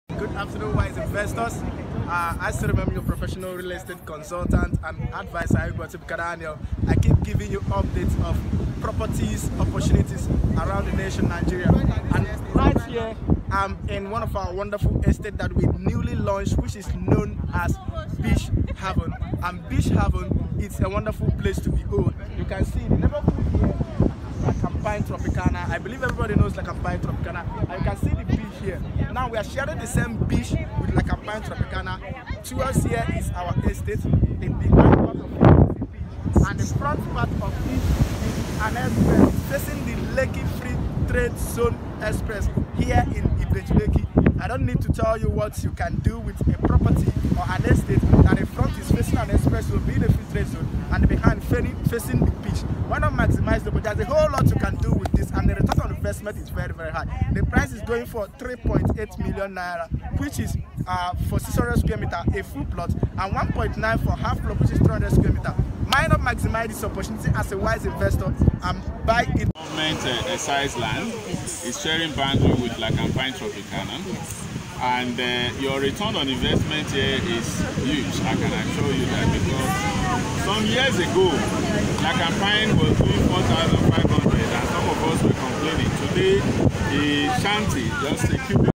How to make high ROI by maximizing investment opportunities in Nigeria
Are you a smart investor?

This property is for you

The front facing the coastal road of Free Trade Zone ibeju lekki and the behind facing the beach.

The commercial value is extremely high

Your ROI is over 1000%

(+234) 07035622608